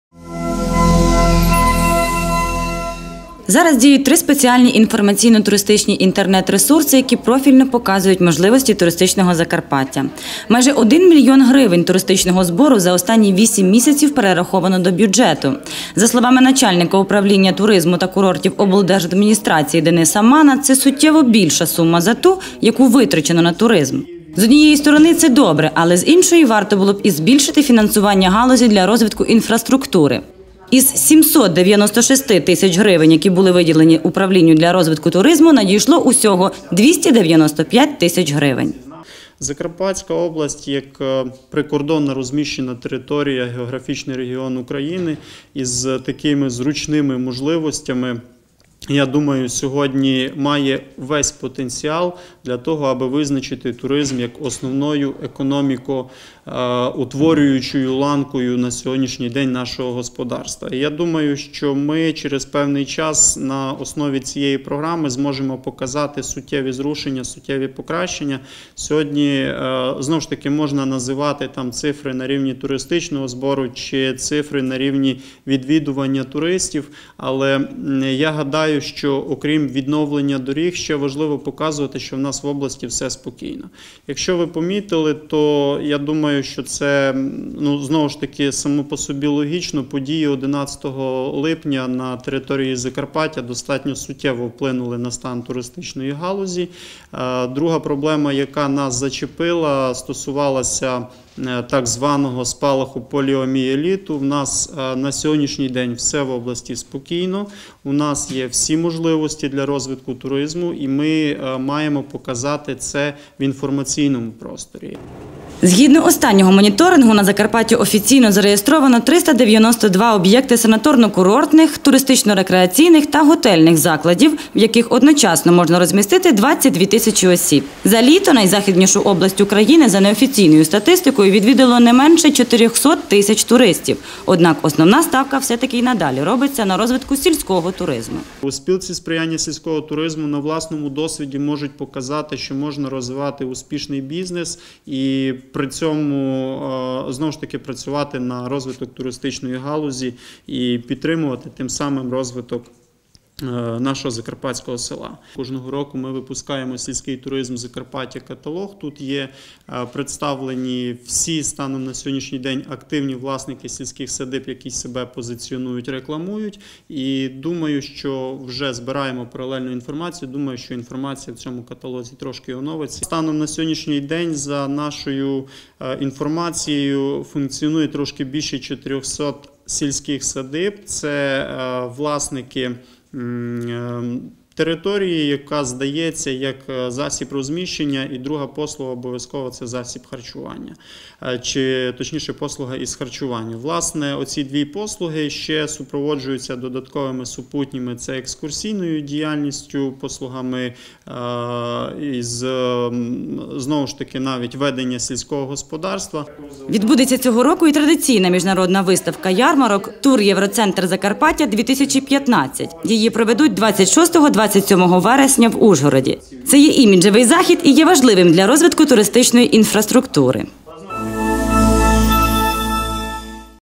20 000 туристів водночас може прийняти Закарпаття
Майже 1 мільйон гривень туристичного збору за останні вісім місяців  перераховано до бюджету. За словами начальника управління туризму та курортів облдержадміністрації Дениса Мана, це суттєво більша сума за ту, яку витрачено на туризм.